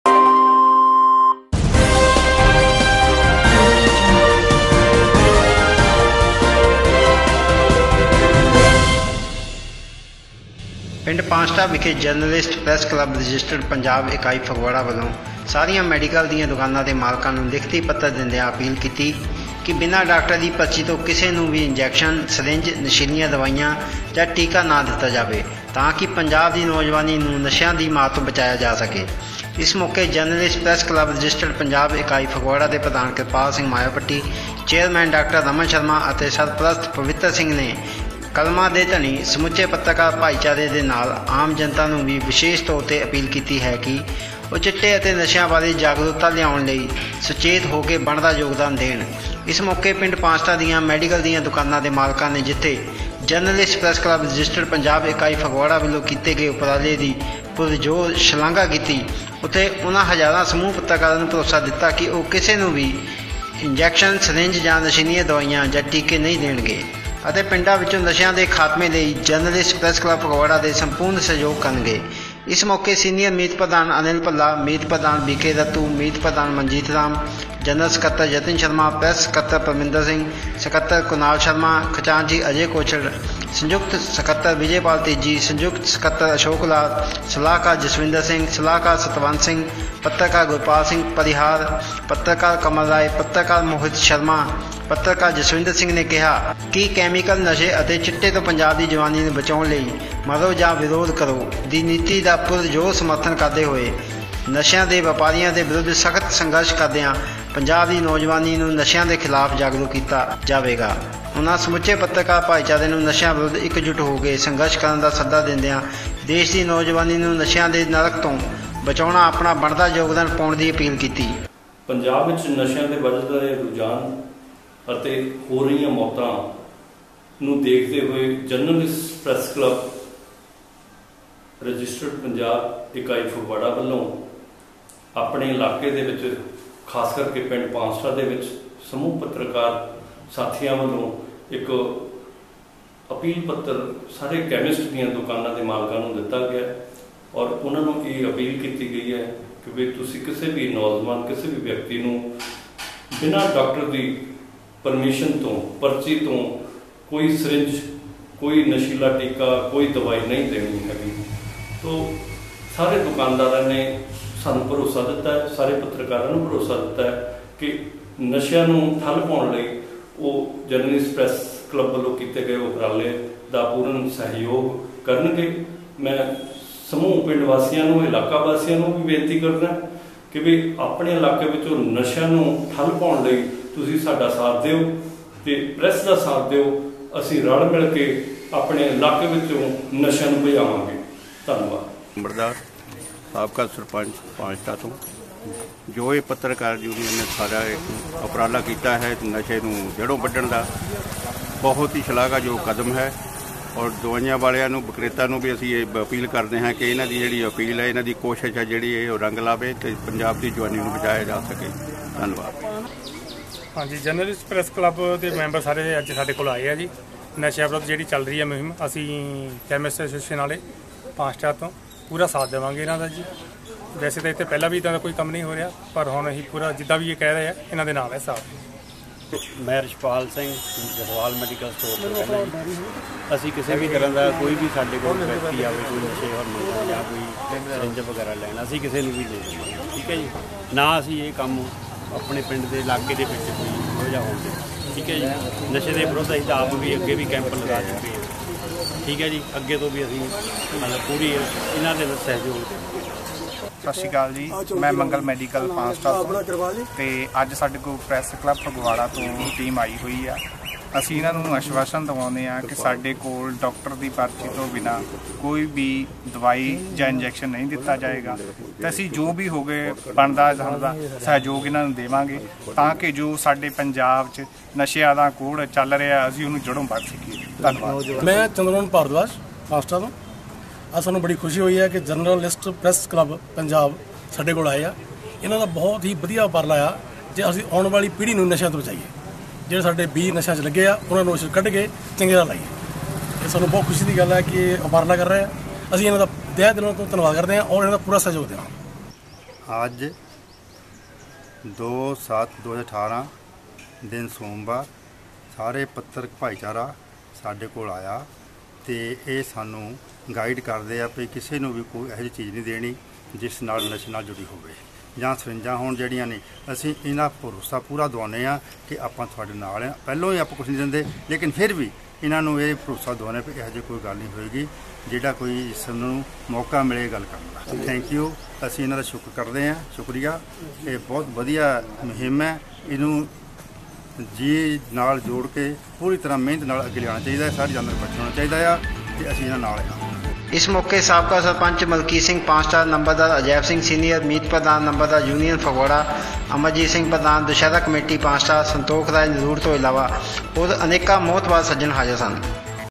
PANCHTA - ” ਮਰੋ ਜਾਂ ਵਿਰੋਧ ਕਰੋ” ਪੰਜਾਬ ਦੀ ਨੌਜਵਾਨੀ ਨੂੰ ਨਸ਼ਿਆਂ ਦੀ ਮਾਰ ਤੋਂ ਬਚਾਇਆ ਜਾ ਸਕੇ
ਪਿੰਡ ਪਾਂਸ਼ਟਾ ਵਿਖੇ ਜਰਨਲਿਸਟ ਪ੍ਰੈਸ ਕਲੱਬ ਰਜਿ:ਪੰਜਾਬ ਇਕਾਈ ਫਗਵਾੜਾ ਵਲੋਂ ਸਾਰੀਆਂ ਮੈਡੀਕਲ ਦੀਆਂ ਦੁਕਾਨਾਂ ਦੇ ਮਾਲਕਾਂ ਨੂੰ ਲਿਖਤੀ ਪੱਤਰ ਦਿੰਦਿਆਂ ਅਪੀਲ ਕੀਤੀ ਪੰਜਾਬ ਦੀ ਨੌਜਵਾਨੀ ਨੂੰ ਨਸ਼ਿਆਂ ਦੀ ਮਾਰ ਤੋਂ ਬਚਾਇਆ ਜਾ ਸਕੇ।